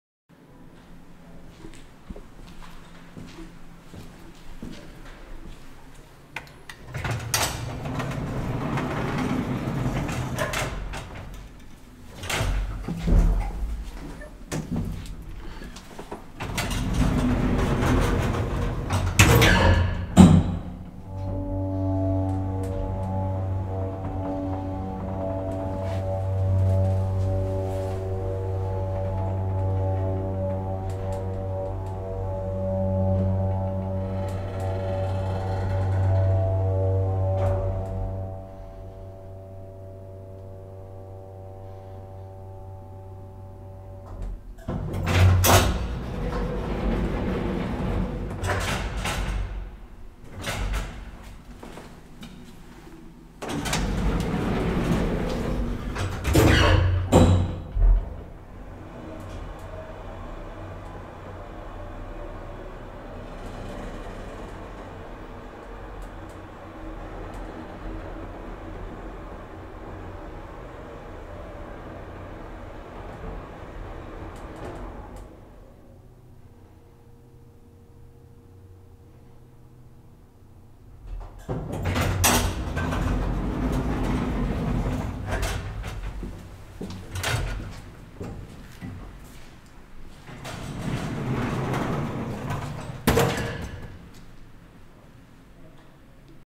EPIC MOTOR!! 1991 KONE M Hydraulic Elevator @ Faxeskolan, Söderhamn
ELEVATOR INFORMATION!

Manufacturer/Tillverkare: KONE
Model/Modell: M
Installation year/Byggår: 1991
Capacity/Maxvikt: 630Kg - 8 Persons
Floors/Antal våningar: 3 = 1 2 3

Location/Adress: "Faxeskolan" - Kungsgatan 13, 826 30 Söderhamn, SWEDEN!